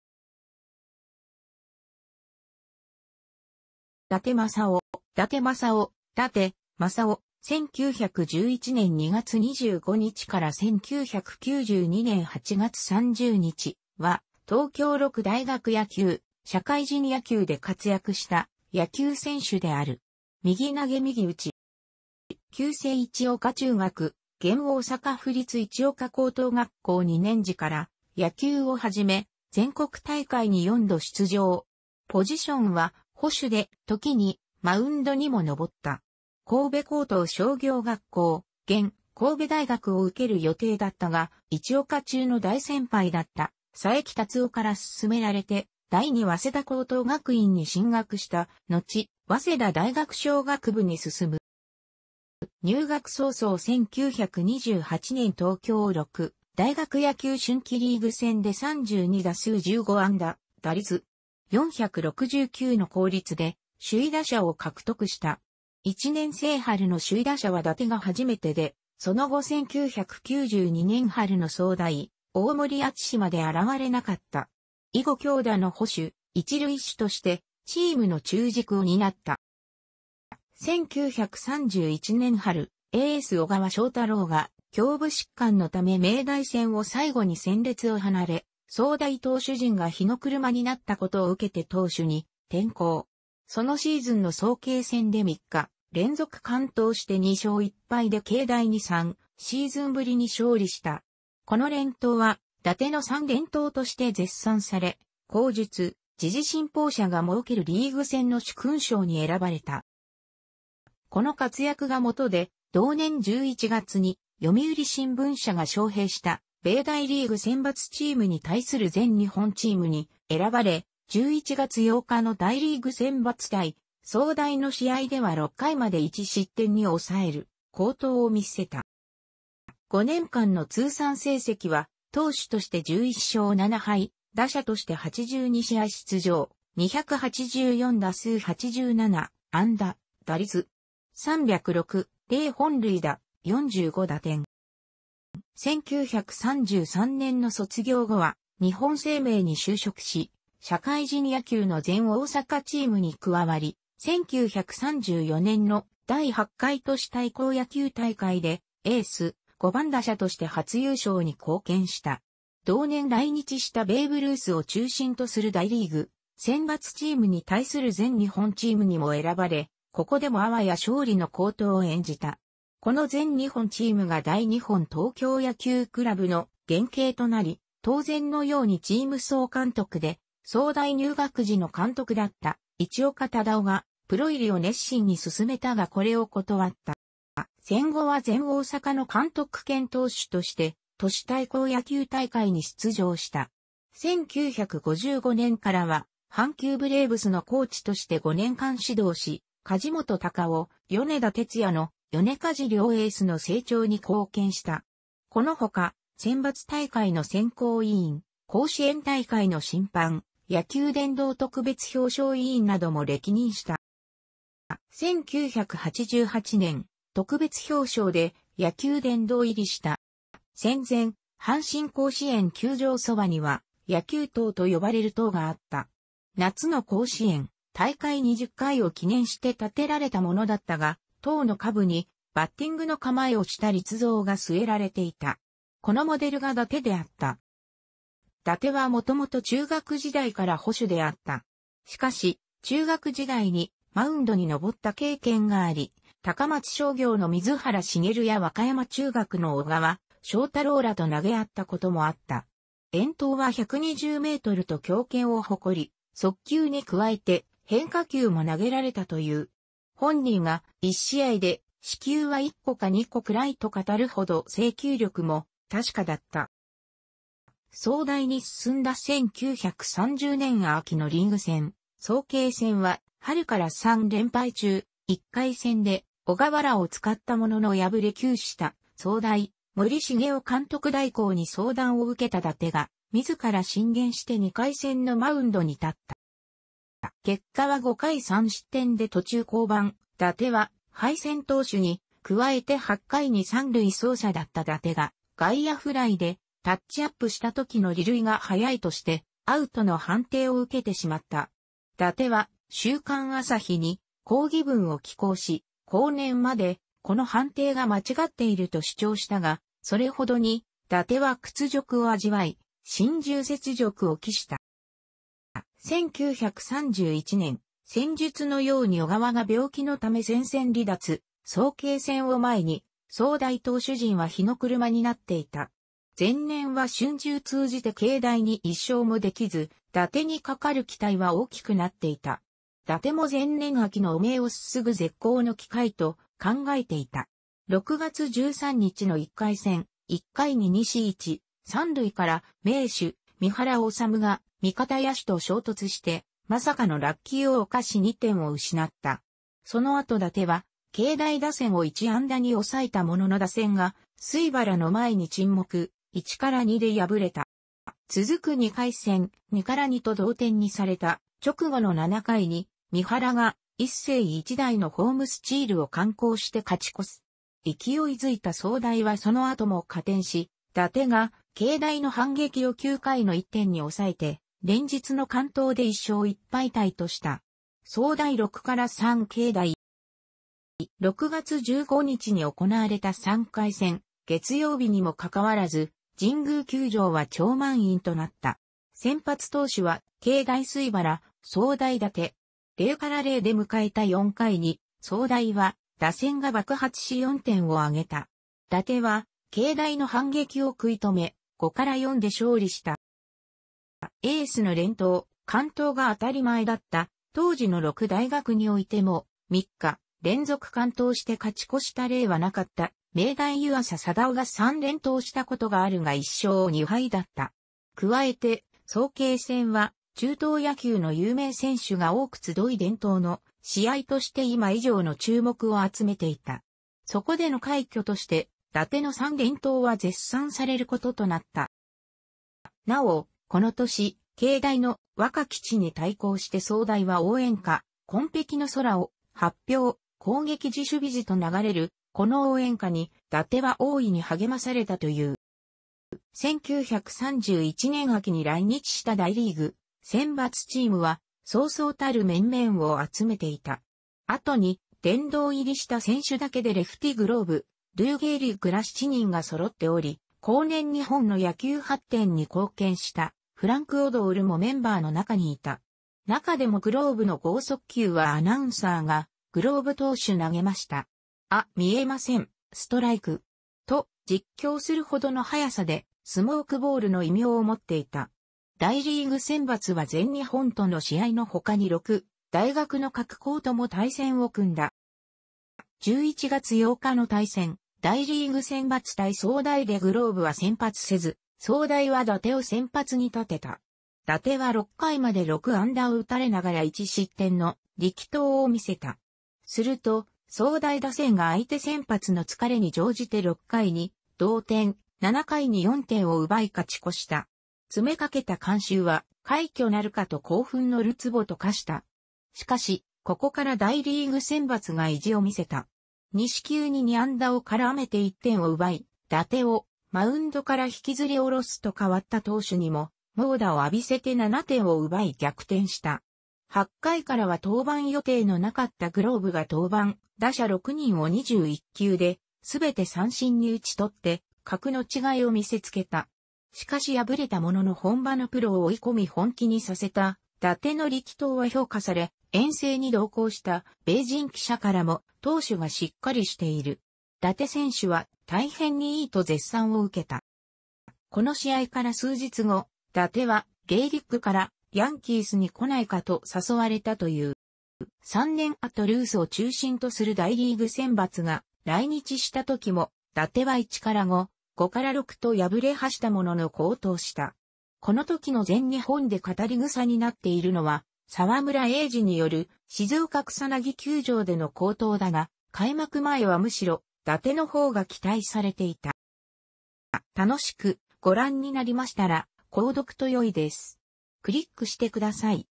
伊達正男
オリックス・バファローズ関連人物

伊達 正男（だて まさお、1911年2月25日 - 1992年8月30日）は、東京六大学野球・社会人野球で活躍した野球選手である。右投右打。

旧制市岡中学（現大阪府立市岡高等学校）2年時から野球を始め、全国大会に4度出場。ポジションは捕手で、時にマウンドにも登った。神戸高等商業学校（現・神戸大学）を受ける予定だったが市岡中の大先輩だった佐伯達夫から薦められて第二早稲田高等学院に進学した（のち早稲田大学商学部に進む）。

入学早々1928年東京六大学野球春季リーグ戦で32打数15安打、打率.469の高率で首位打者を獲得した。1年生春の首位打者は伊達が初めてで、その後1992年春の早大・大森篤まで現れなかった。以後強打の捕手・一塁手としてチームの中軸を担った。

1931年春、エース小川正太郎が胸部疾患のため明大戦を最後に戦列を離れ、早大投手陣が火の車になったことを受けて投手に転向。そのシーズンの早慶戦で3日連続完投して2勝1敗で慶大に3シーズンぶりに勝利した。この連投は伊達の三連投として絶賛され（後述）、時事新報社が設けるリーグ戦の殊勲賞に選ばれた。
この活躍がもとで同年11月に読売新聞社が招聘した米大リーグ選抜チームに対する全日本チームに選ばれ、11月8日の大リーグ選抜対早大の試合では6回まで1失点に抑える好投を見せた。

5年間の通算成績は投手として11勝7敗、打者として82試合出場、284打数87安打、打率.306、0本塁打、45打点。

1933年の卒業後は日本生命に就職し、社会人野球の全大阪チームに加わり、1934年の第8回都市対抗野球大会でエース・五番打者として初優勝に貢献した。同年来日したベーブ・ルースを中心とする大リーグ選抜チームに対する全日本チームにも選ばれここでもあわや勝利の好投を演じた。この全日本チームが大日本東京野球倶楽部の原型となり、当然のようにチーム総監督で早大入学時の監督だった市岡忠男がプロ入りを熱心に勧めたがこれを断った。

戦後は全大阪の監督兼投手として都市対抗野球大会に出場した。1955年からは阪急ブレーブスのコーチとして5年間指導し、梶本隆夫・米田哲也の「ヨネ・カジ」両エースの成長に貢献した。このほか選抜大会の選考委員、甲子園大会の審判、野球殿堂特別表彰委員なども歴任した。

1988年、特別表彰で野球殿堂入りした。

戦前、 阪神甲子園球場そばには「野球塔」と呼ばれる塔があった。夏の甲子園大会20回を記念して建てられたものだったが、塔の下部にバッティングの構えをした立像が据えられていた。このモデルが伊達であった。

伊達はもともと中学時代から捕手であった。しかし中学時代にマウンドに上った経験があり、高松商業の水原茂や和歌山中学の小川正太郎らと投げ合ったこともあった。遠投は120mと強肩を誇り、速球に加えて変化球も投げられたという。本人が「1試合で四球は1個か2個くらい」と語るほど制球力も確かだった。
早大に進んだ1930年秋のリーグ戦、早慶戦は春から3連敗中、1回戦で小川らを使ったものの敗れ窮した早大・森茂雄監督代行に相談を受けた伊達が、自ら進言して2回戦のマウンドに立った。

結果は5回3失点で途中降板（伊達は敗戦投手に）、加えて8回に三塁走者だった伊達が外野フライでタッチアップしたときの離塁が早いとしてアウトの判定を受けてしまった。伊達は『週刊朝日』に抗議文を寄稿し、後年までこの判定が間違っていると主張したが、それほどに伊達は屈辱を味わい、心中雪辱を期した。

1931年、先述のように小川が病気のため戦線離脱、早慶戦を前に早大投手陣は火の車になっていた。前年は春秋通じて慶大に1勝もできず、伊達にかかる期待は大きくなっていた。伊達も前年秋の汚名を雪ぐ絶好の機会と考えていた。
6月13日の1回戦、1回に2死1､3塁から名手三原脩が味方野手と衝突してまさかの落球をおかし2点を失った。その後伊達は慶大打線を1安打に抑えたものの打線が水原の前に沈黙、1-2で敗れた。

続く2回戦、2-2と同点にされた直後の7回に三原が一世一代のホームスチールを敢行して勝ち越す。勢いづいた早大はその後も加点し、伊達が慶大の反撃を9回の1点に抑えて連日の完投で1勝1敗のタイとした。早大6-3慶大。

6月15日に行われ...